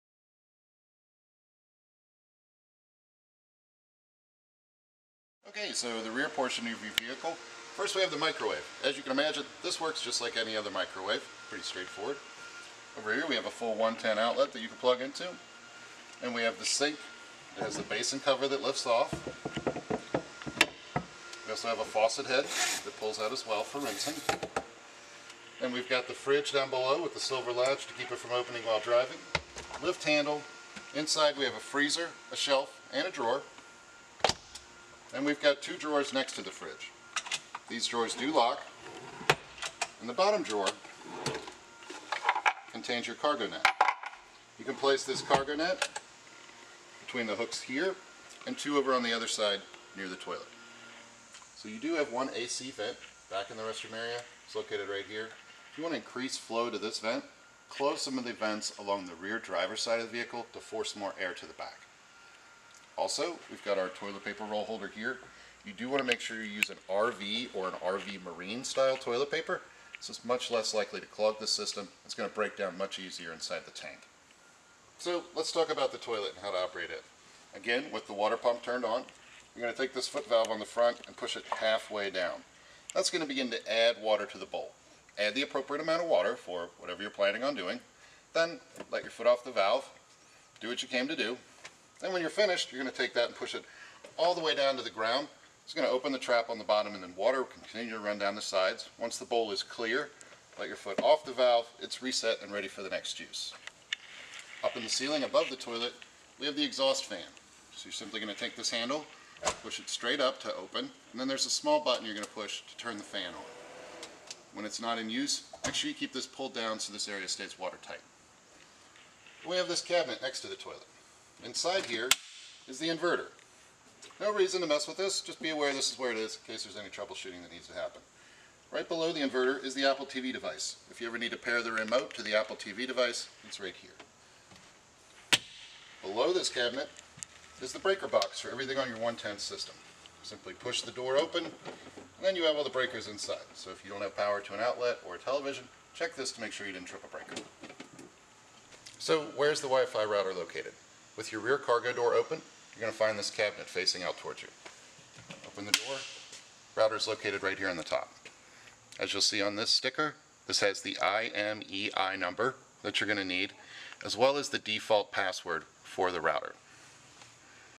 The Ultimate Coach - Bathroom, Kitchen and Router
This is a guide to your new Ultimate Coach's bathroom, kitchen and router.

Follow us for more tips and tricks, along with details on our newest luxury vehicles.